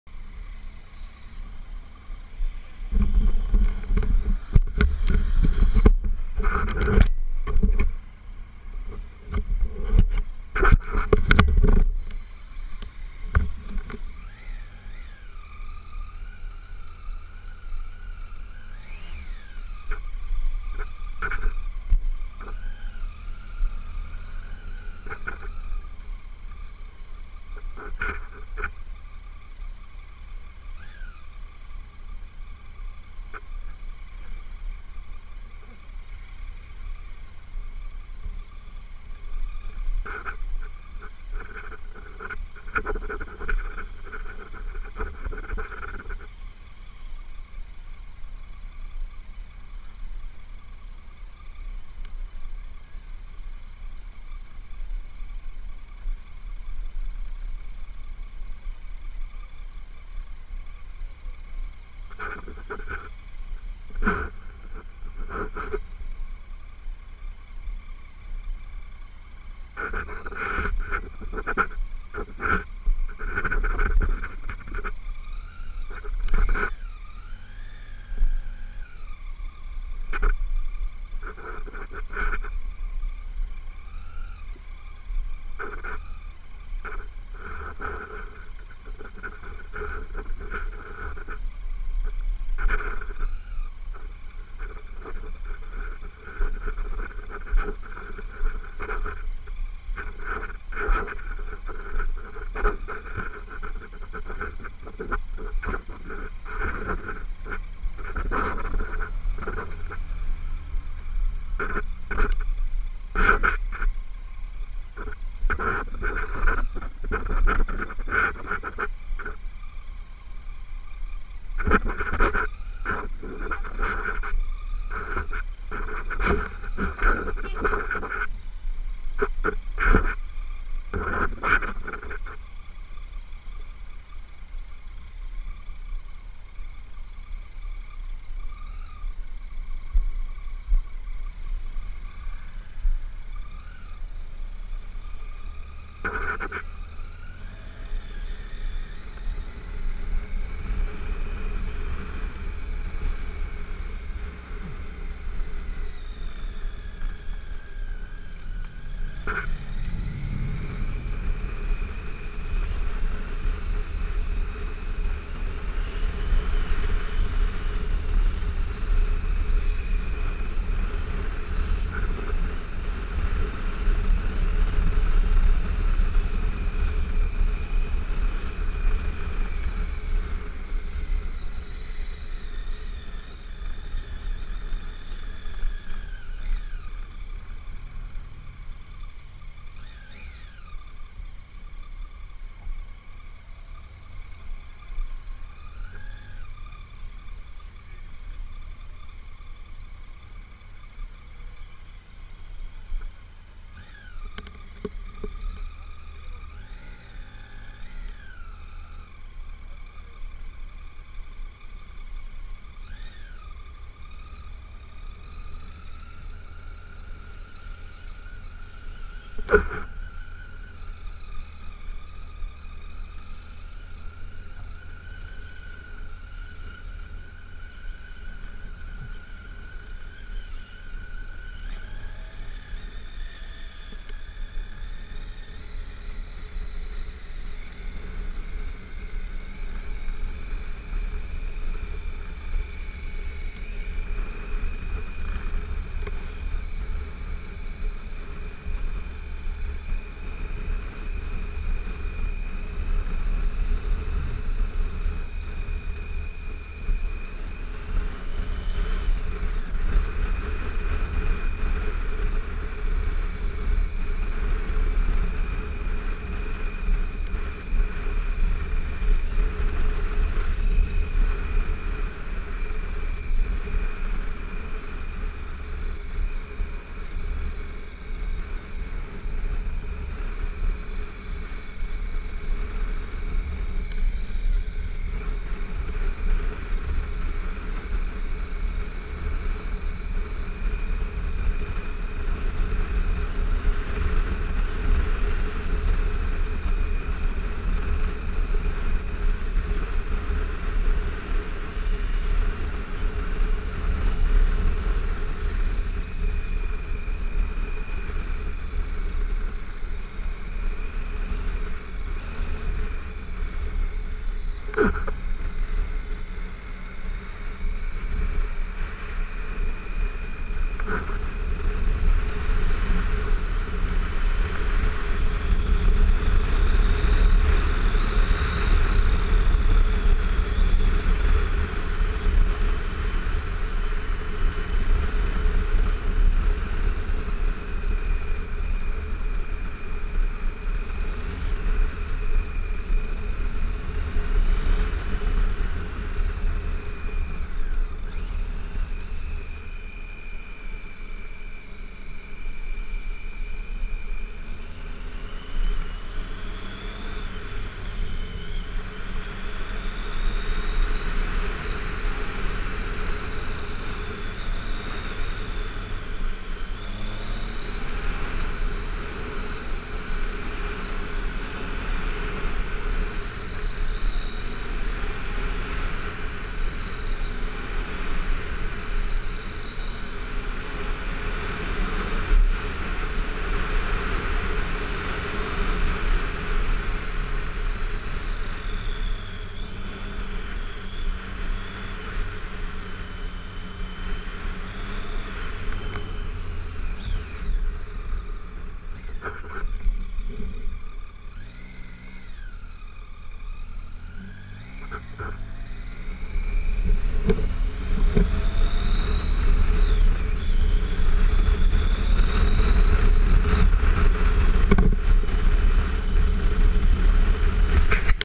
Tiger 800 ANFAD
Ankara Off-Road Şampiyonası’nın 3. ve son ayağı